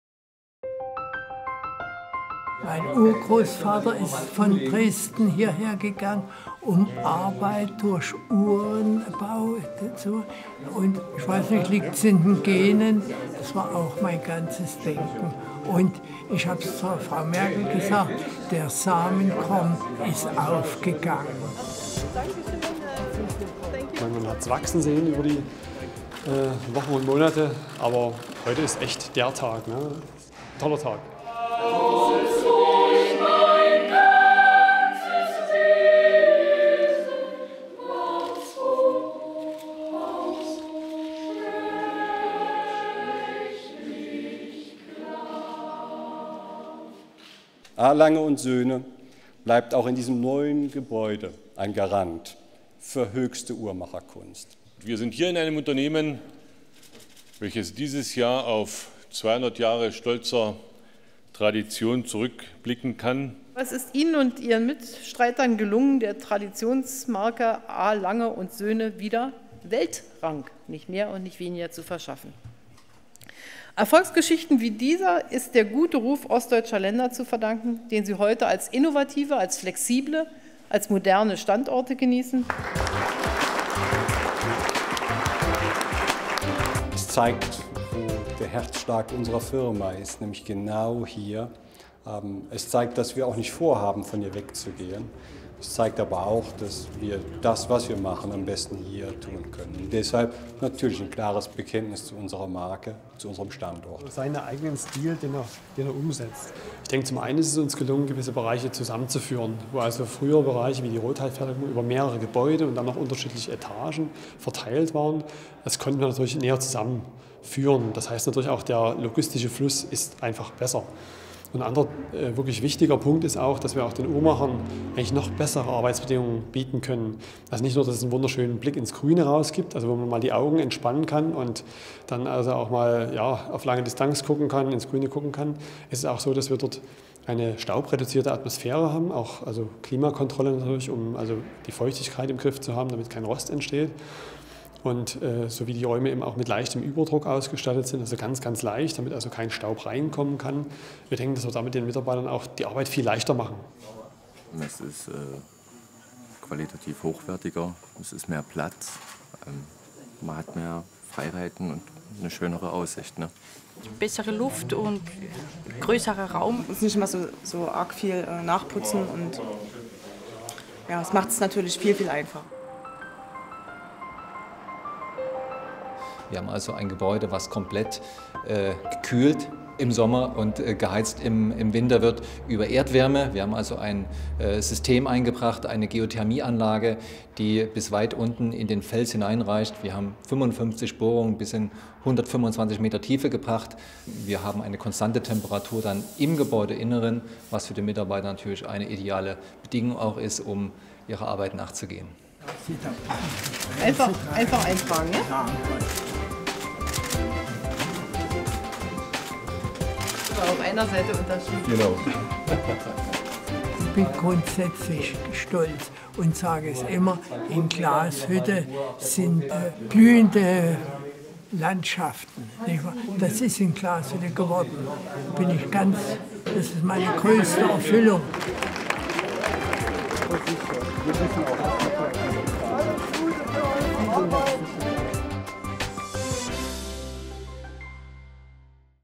A. Lange & Söhne - Inauguration of the new building
Inauguration of the new factory Building on 26 August, 2015.